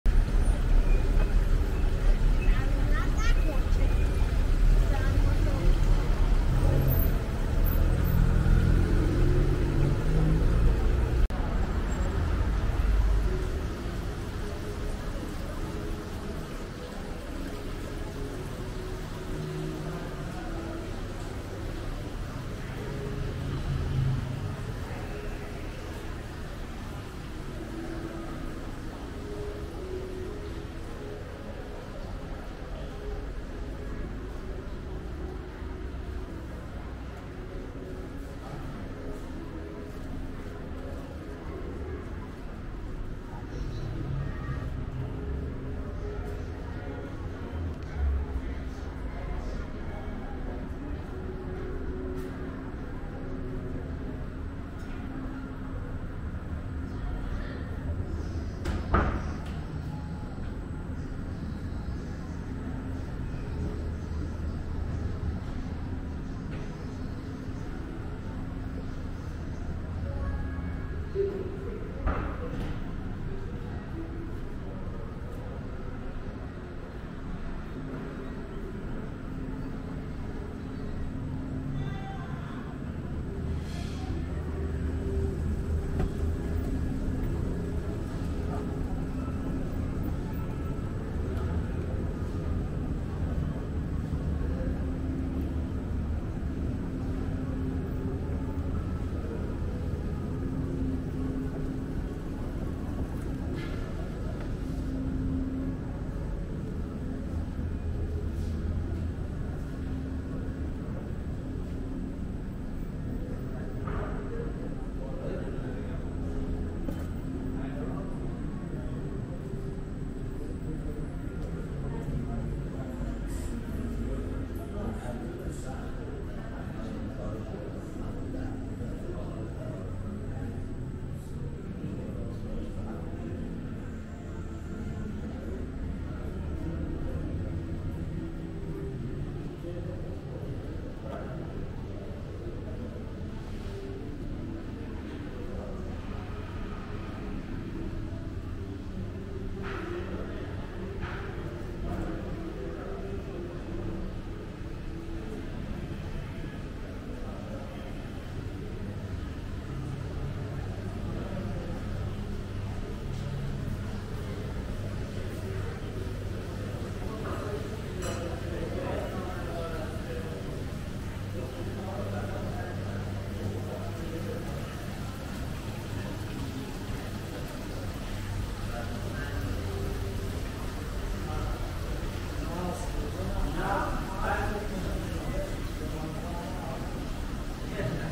Tehran, Iran 2022 - Walking In Queen Center | Luxury Mall In Tehran Walking tour
Queen Center (Bazaar and Shopping Center) is located in Elahieh neighborhood of Tehran and Maryam Gharbi Street. This complex is one of the thirteen markets and shopping centers in Elahieh neighborhood of Tehran and is geographically close to Elahieh Modern Shopping Center as well as Royal Address Complex, Food Hall, Vicolo Restaurant Cafe, Diamond Hotel and Turkish Airlines.

Walking in Tehran - Video Walk, Video Walk Iran 4K and Walking tour Tehran.
Visiting Iran, what to see and do?